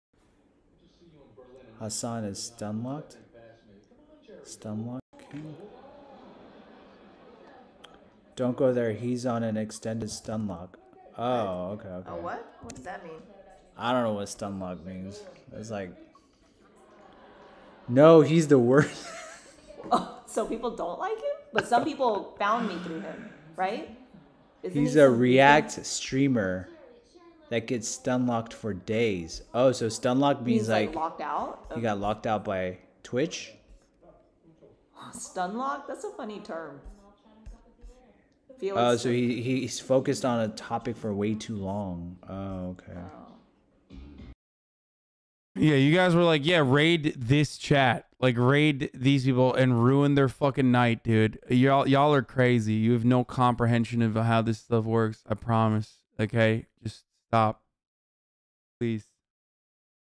Hasanabi Reacting to Christine (From Masterchef) Reacting to Him
widepeepoHappy